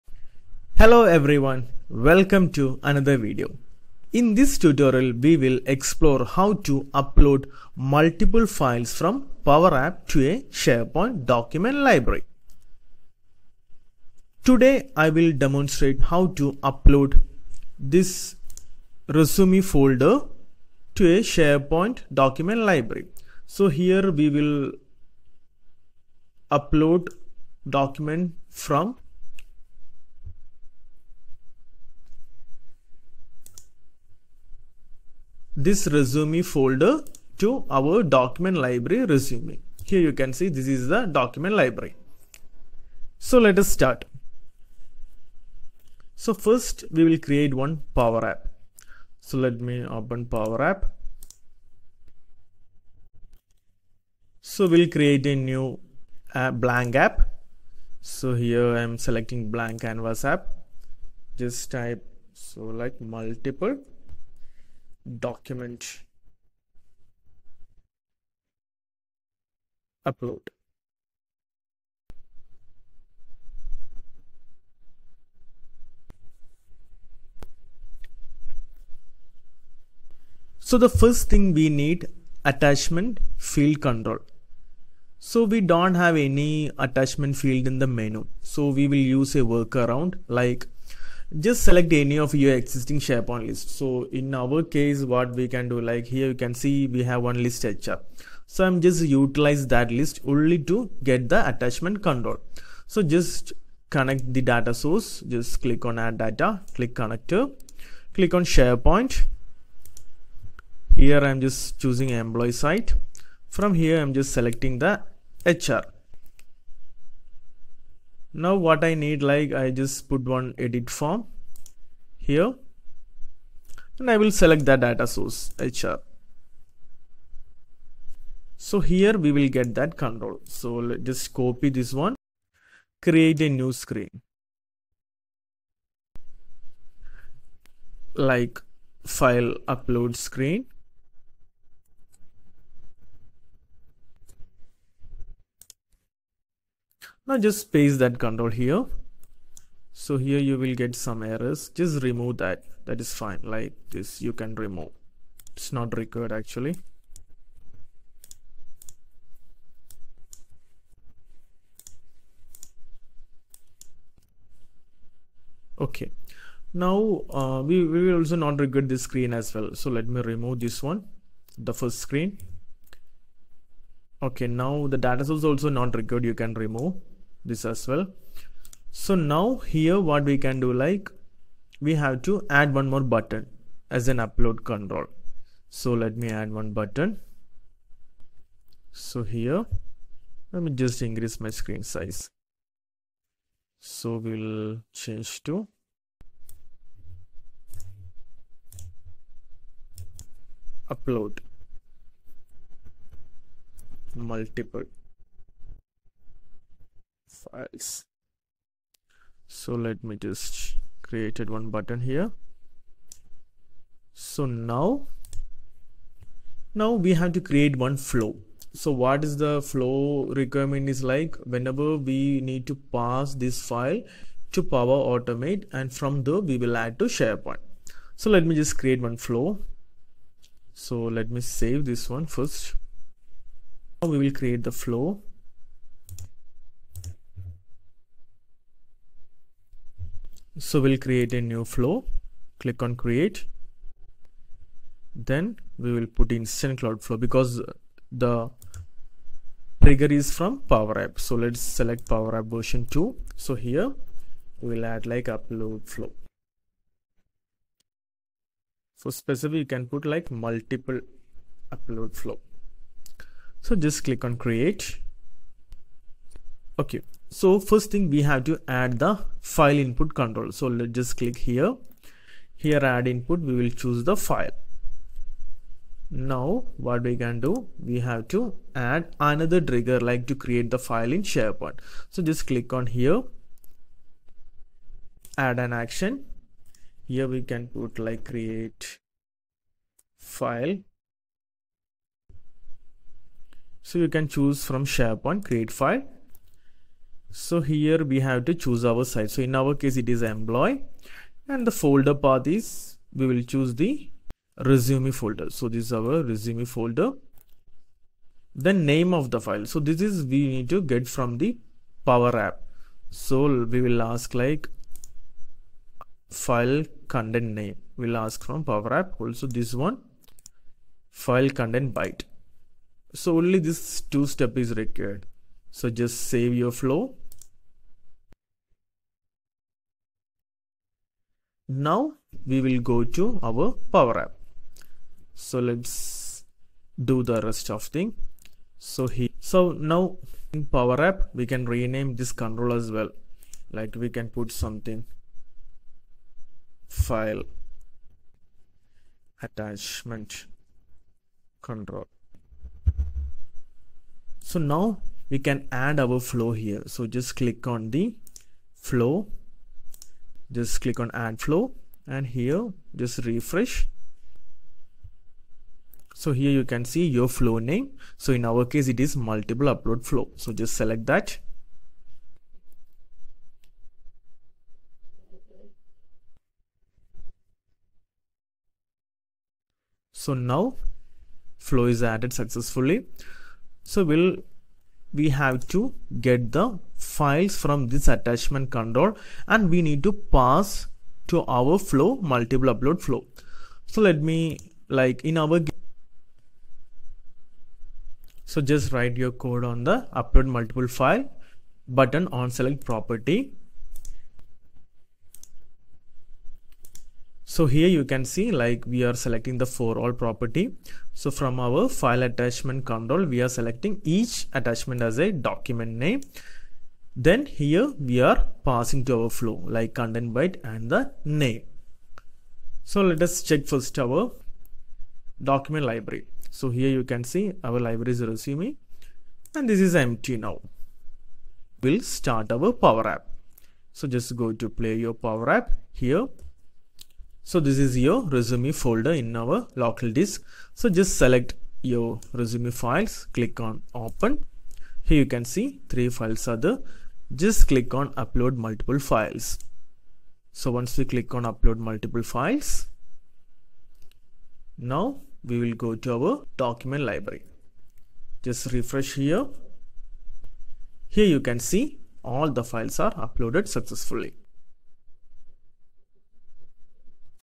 Multiple Documents: Uploading Multiple Files to SharePoint Document Library from PowerApps
In this video, you will learn the step-by-step process of uploading multiple files to a SharePoint Document Library from PowerApps.

ForAll function (Syntax):
ForAll(Table, Formula)

Actions:
Create file : Uploads a file to a SharePoint site. Make sure to pick an existing library.